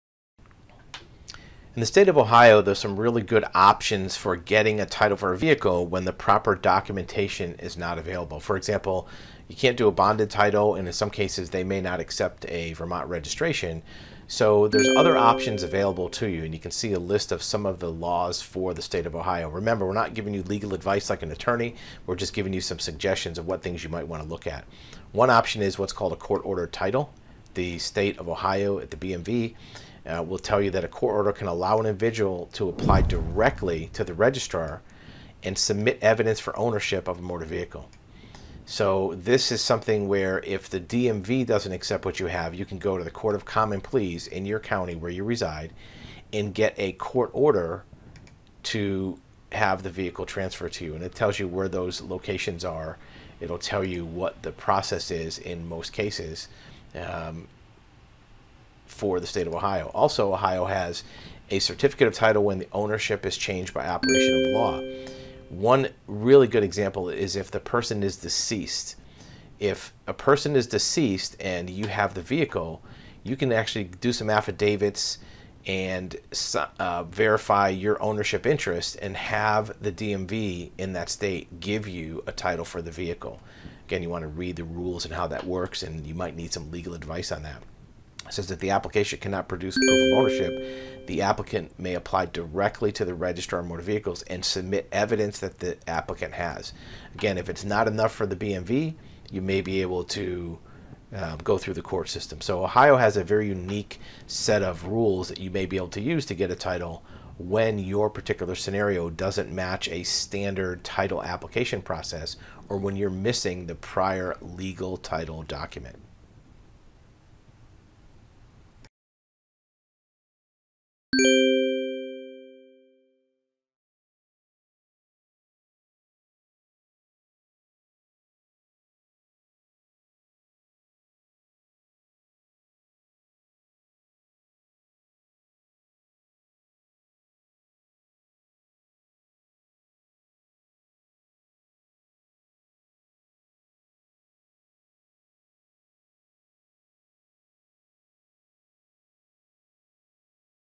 Ohio car title options for difficult situations
How to get a vehicle title in Ohio when the complete documents are not available, using statutory methods or court ordered processes